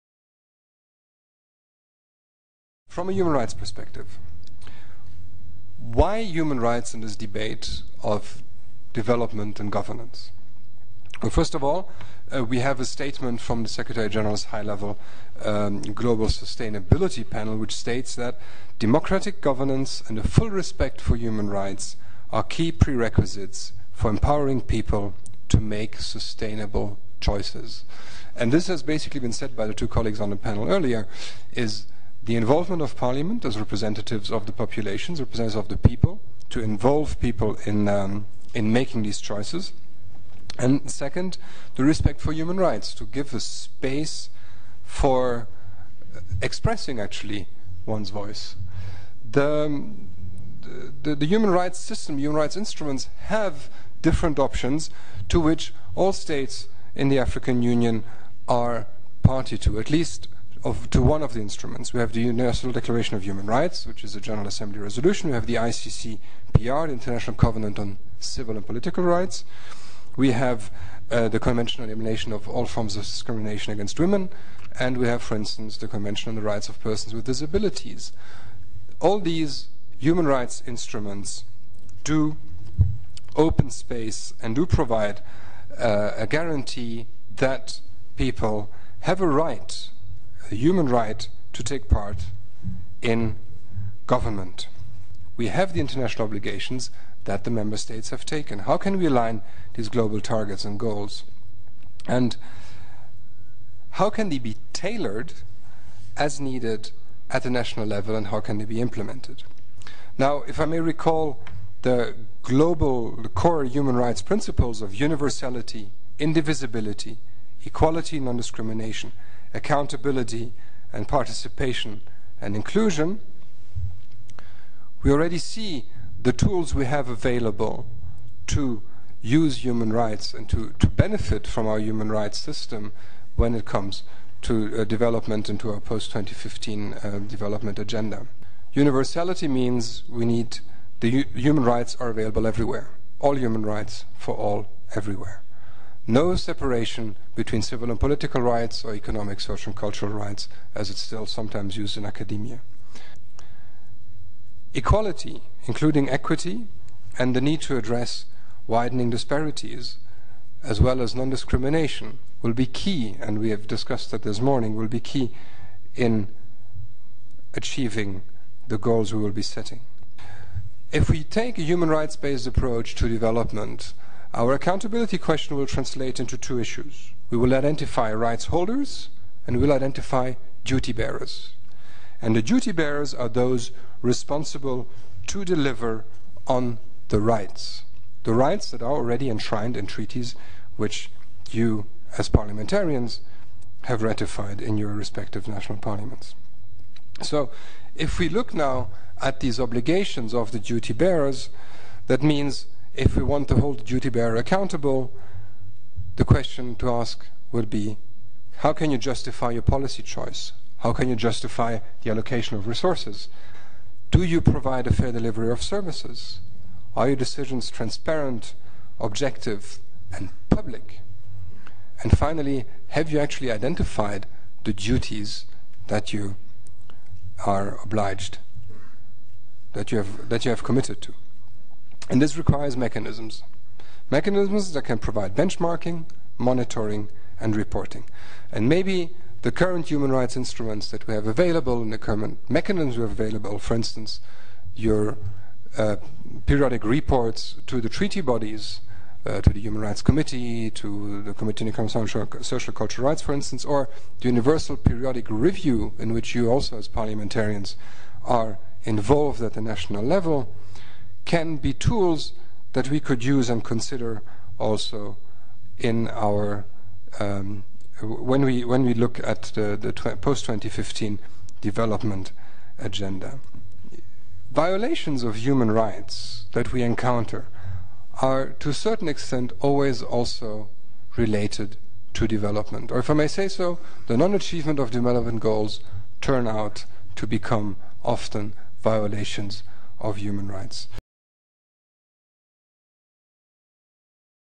Marco Kalbusch, OHCHR - African Regional Dialogue on Governance and Post-2015
African Regional Dialogue - The New Development Agenda: Post-2015 Global Thematic Consultation on Governance, 11-12 October 2012, Johannesburg, South Africa

Plenary Session 1 - Intervention by Marco Kalbusch, Human Rights Officer and Deputy Regional Representative, OHCHR